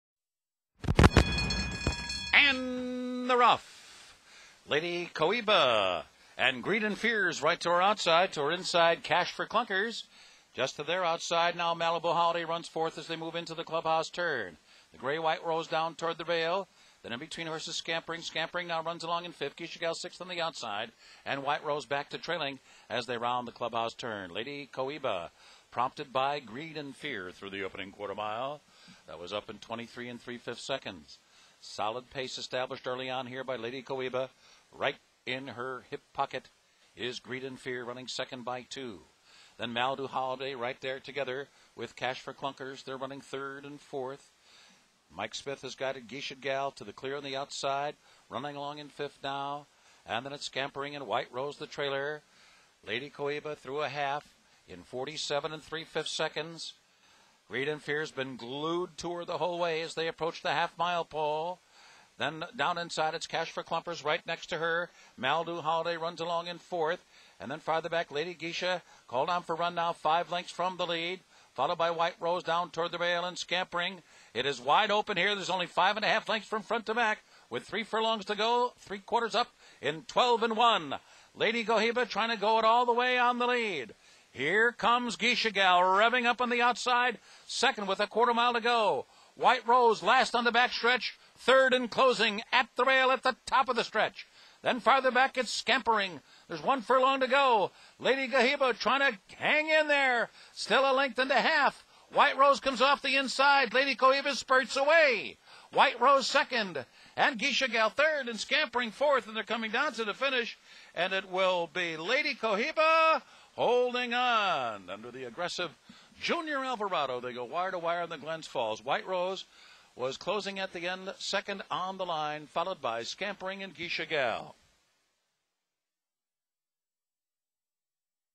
Lady Cohiba - 2013 Glens Falls (G3)
Confidently ridden by Junior Alvarado, favored Lady Cohiba took the lead coming out of the gate and held on for a half-length victory over White Rose in the $150,000 Glens Falls, one of two stakes on Monday's closing-day card at Saratoga Race Course.